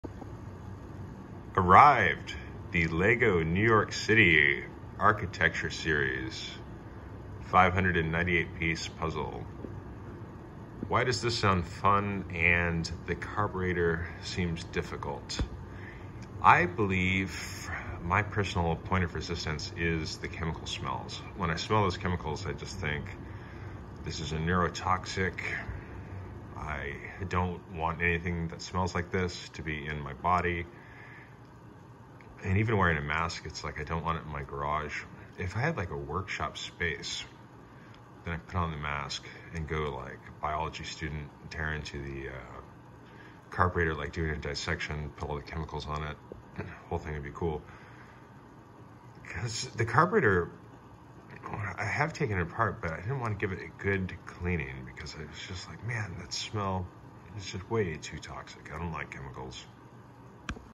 Lego Architecture New York City 21028 | Harley Davidson Carburetor. Why does one seem more doable
Lego set arrived in ok shape, typical of Amazon, if you truly are collecting the box, it will arrived already damaged :( . Great price though and looking for the build more than the box however i appreciate a new looking box.  In store, IRL is probably the only way to get a box, pick the one from the back etc. one that doesn't have shelf wear...